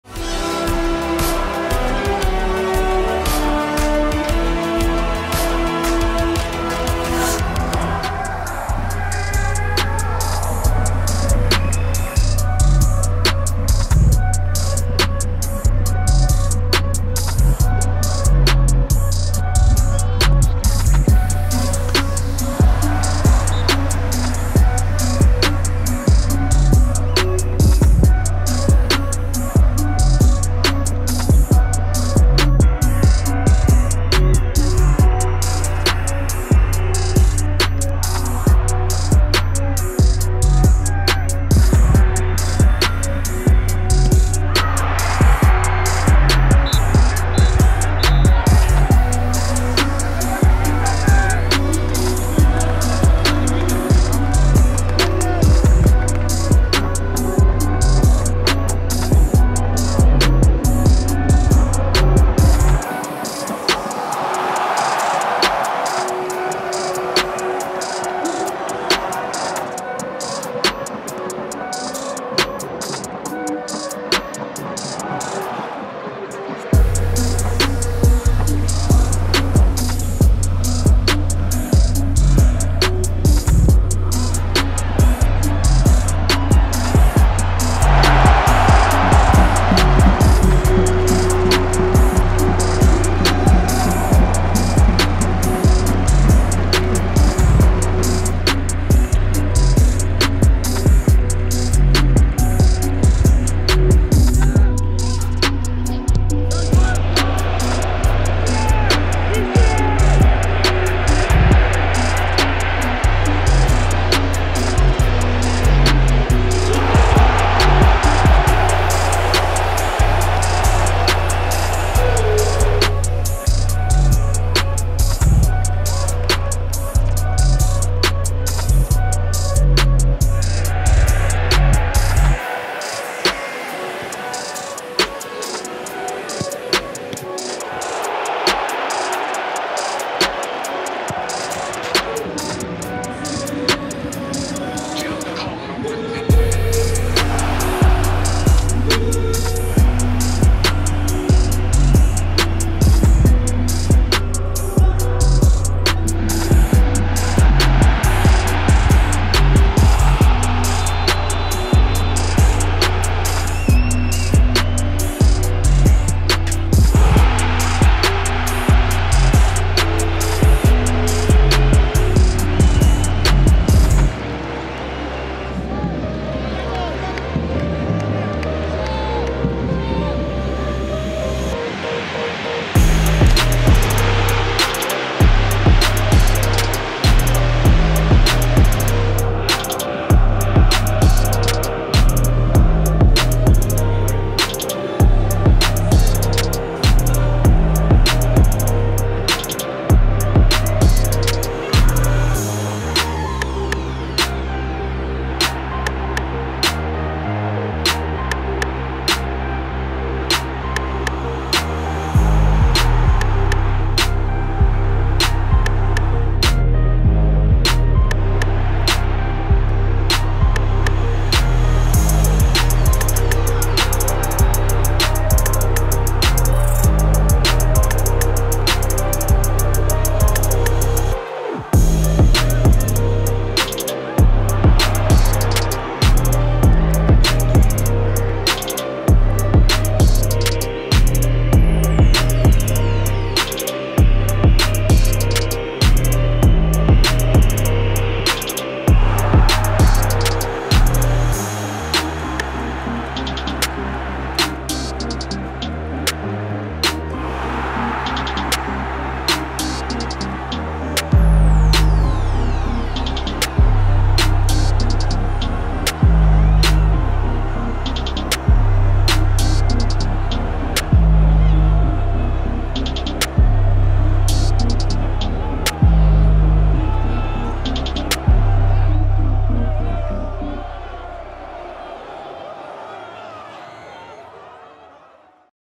Le parcours du FC Nantes en Coupe de France 2021-2022
Revivez le parcours du FC Nantes jusqu'à la finale de la Coupe de France. Les canaris affronteront l’OGC Nice ce samedi 7 mai au Stade de France (dès 20h45 sur Eurosport 2 et France 2).